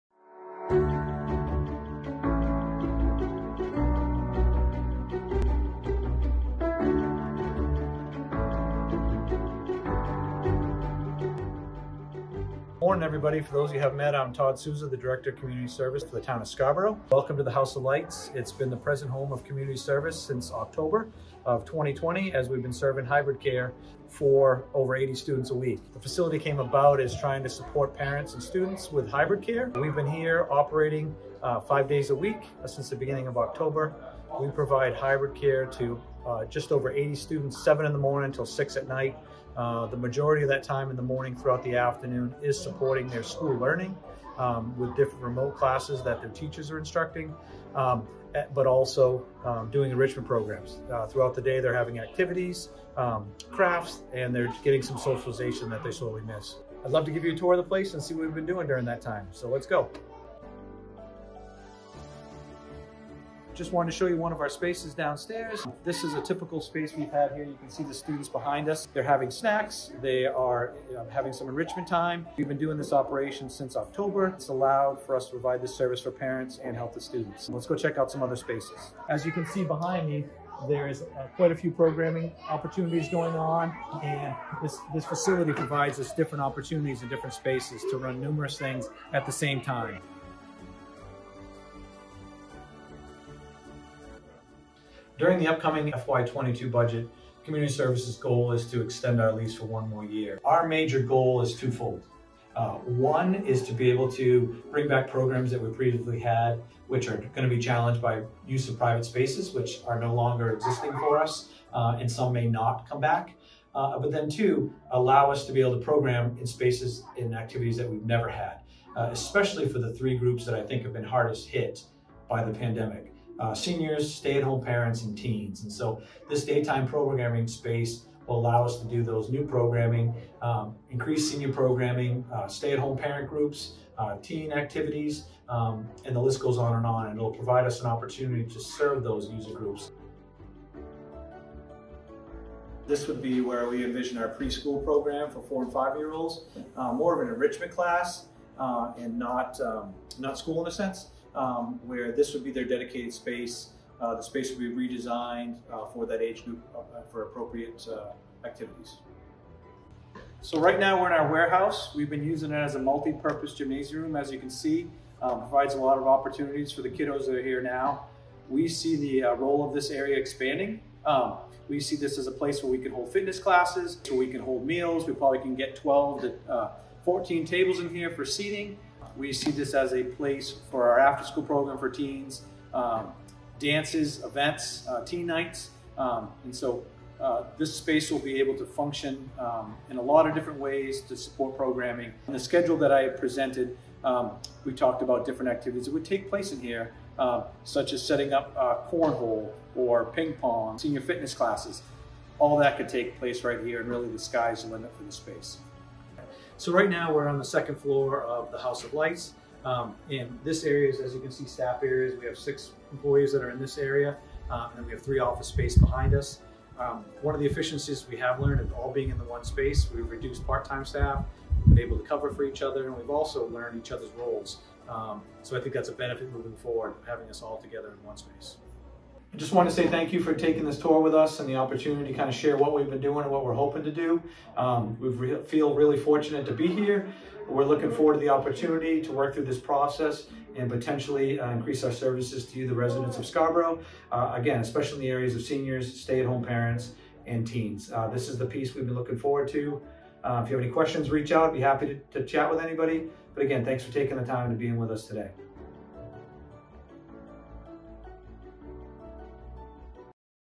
Take a Tour of our Community Services Hub!
The Town of Scarborough Community Services department moved their offices from Town Hall to the House of Lights building on 418 Payne Road in October 2020. This also centralized program offerings and school support programs to a single location, now referred to as the Hub.

As part of the Fiscal Year 2022 budget, Community Services is proposing to re-lease this space for the year ahead. Learn more from Community Services Director Todd Souza on the opportunities that could result from the continued use of this space.

Read more on the Hub proposed use and programming schedule on page 143 of the FY2022 Budget Document on the Town of Scarborough Budget Portal